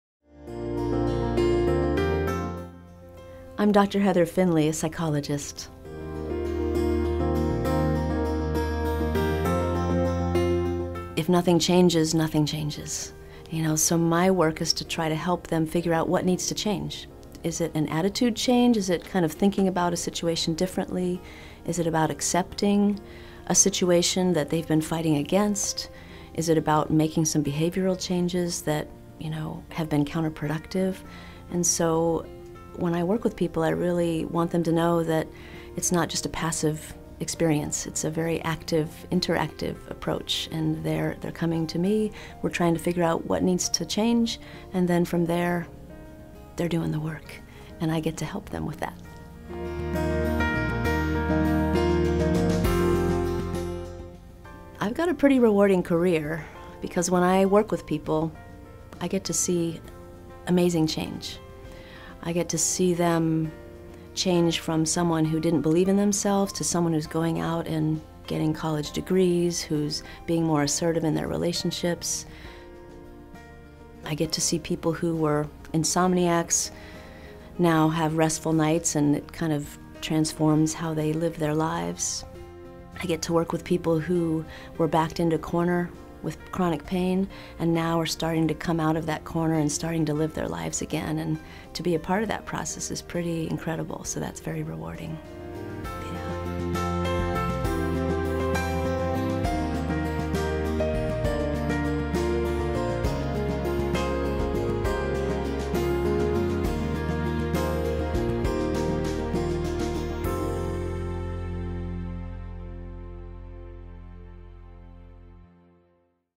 Heather C. Finley, PhD, Psychologist, University of Vermont Medical Center
Dr. Heather Finley is a psychologist at the University of Vermont Medical Center. She treats patients with chronic pain, insomnia, and anxiety. Dr. Finley specializes in Cognitive-Behavioral Therapy (CBT), Acceptance and Commitment Therapy (ACT), positive psychology, mindfulness, clinical hypnosis, and Eye Movement Desensitization and Reprocessing (EMDR).